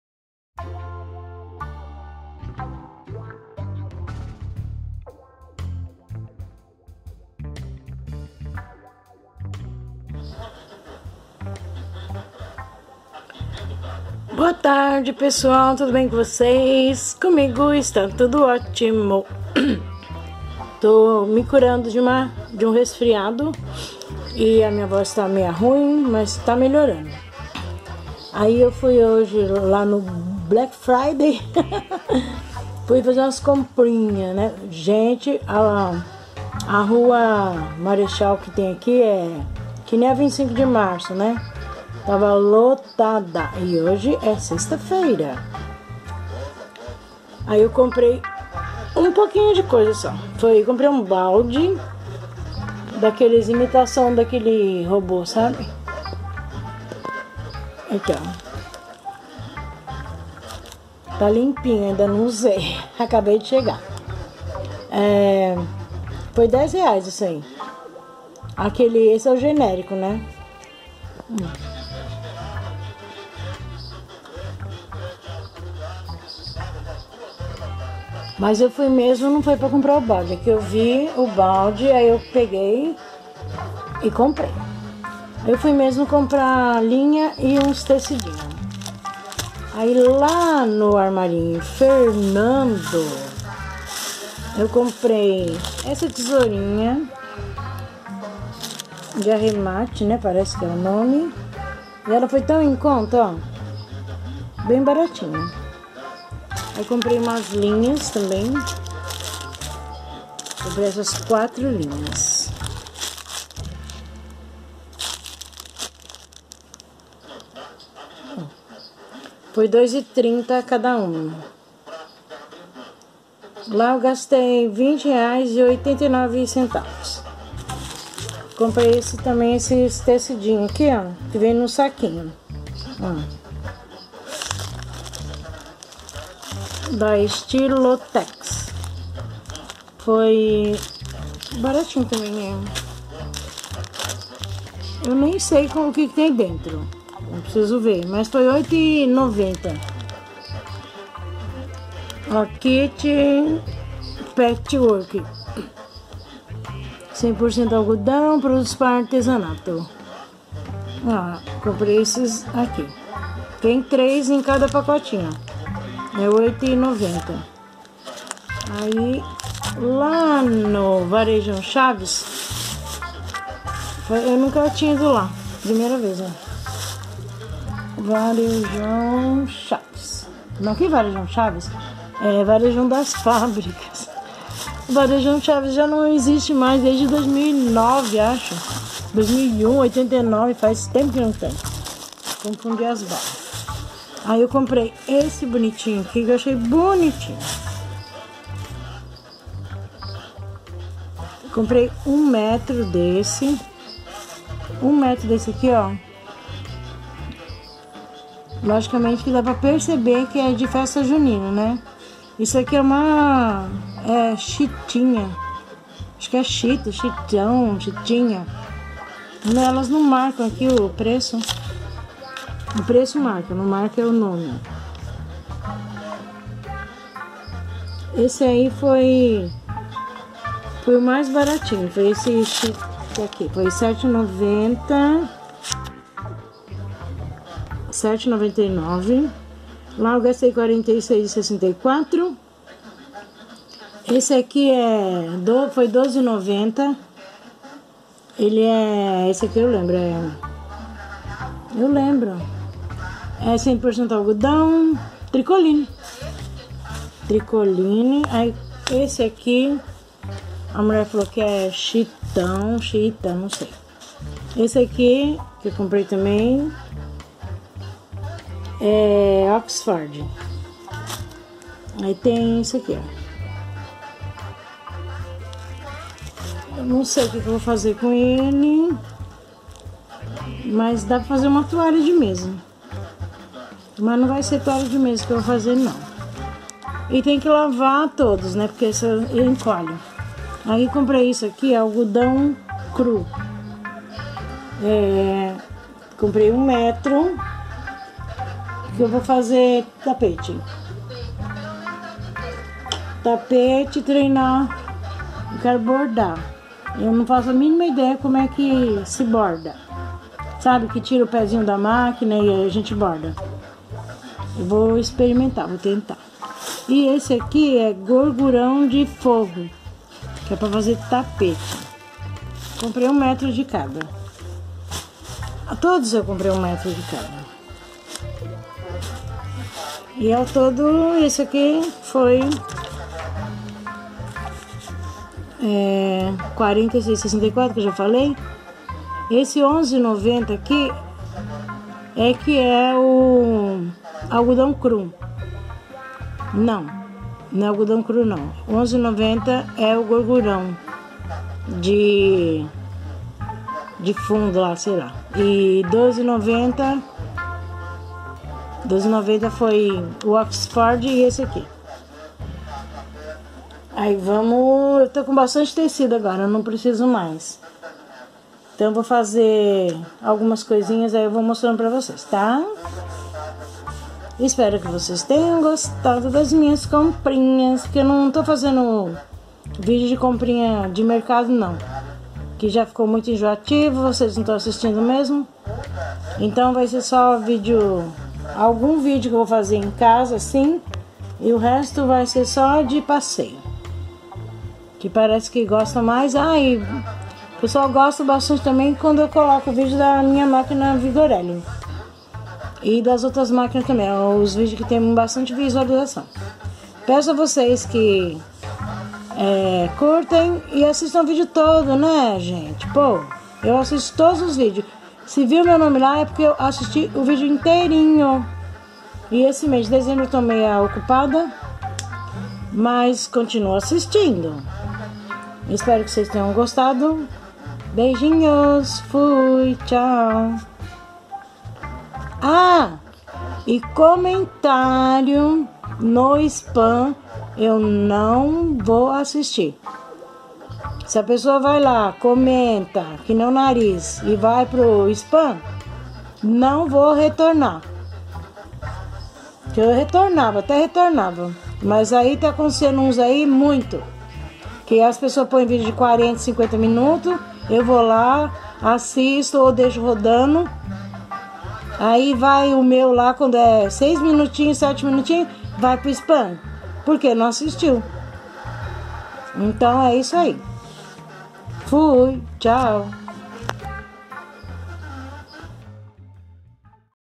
Comprinhas no Varejão das Fábricas
Minhas comprinhas no Black Friday

Comprinhas de tecidos no varejão,os preços estavam bons, comprei também linhas e uma tesoura de arremate que eu estava muito querendo ter,paguei muito baratinho,pq eu vi no mercado livre por mais de 20 reais com frete fica mais caro,assistam o vídeo e confira como foi em conta.